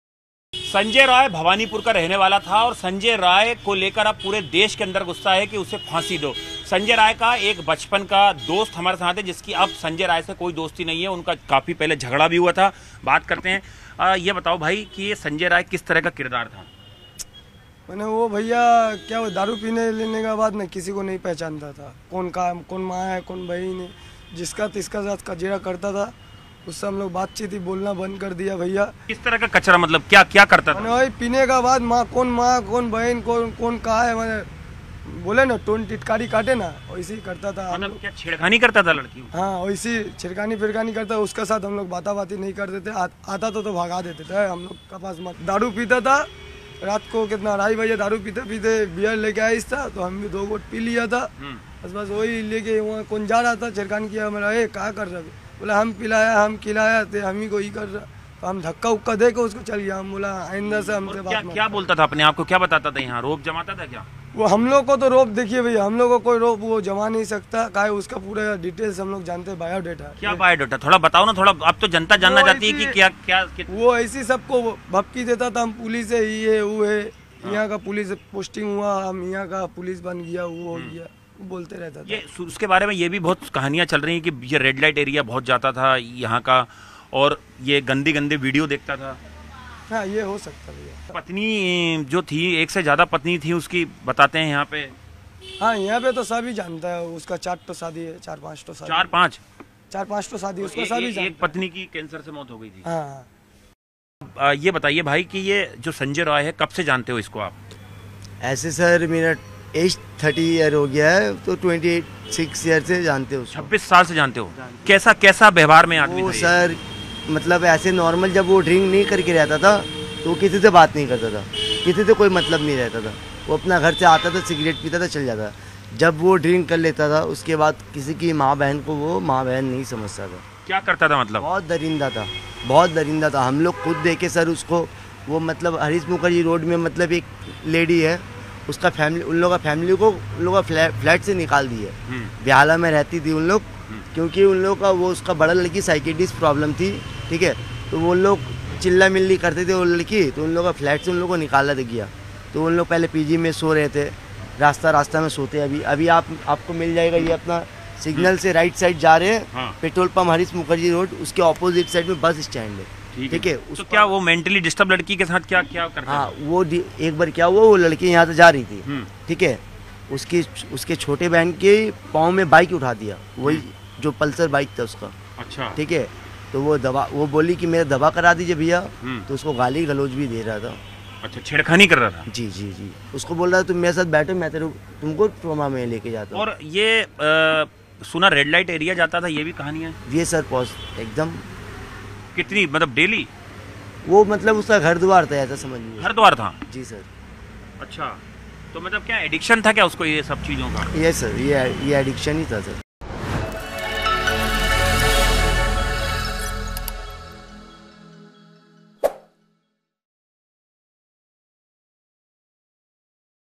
Kolkata Doctor Case: कोलकाता कांड के आरोपी संजय रॉय बारे Friends का हैरतअंगेज खुलासा | RG Kar | Top
The things that have come to light so far in the CBI investigation regarding Sanjay Roy, the main accused in the murder case of Lady Doctor in Kolkata, are extremely shocking. The claim made by Sanjay Roy's friends about him is even more shocking. His friends, who knew Sanjay Roy for 26 years, told that after drinking alcohol, Sanjay used to behave like a wolf. He also kept an evil eye on his friends' sisters. He told that drinking alcohol and teasing girls were Sanjay's habits. Along with this, he also used to beat his wife badly.

कोलकाता में लेडी डॉक्टर की मर्डर केस के मुख्य आरोपी संजय रॉय को लेकर सीबीआई की जांच में अब तक जो बातें सामने आई हैं, वह बेहद चौंकाने वाली हैं. वहीं संजय रॉय के दोस्तों ने उसे लेकर जो दावा किया है, वह और भी ज्यादा चौंकाने वाला है. संजय रॉय को 26 साल से जानने वाले उसके दोस्तों ने बताया कि शराब पीने के बाद संजय भेड़िए जैसा व्यवहार करता था. दोस्तों की बहनों पर भी बुरी नजर रखता था. उन्होंने बताया कि शराब पीना और लड़कियां छेड़ना संजय की आदत थी. इसके साथ ही वह अपनी पत्नी को भी बुरी तरह मारता-पीटता था.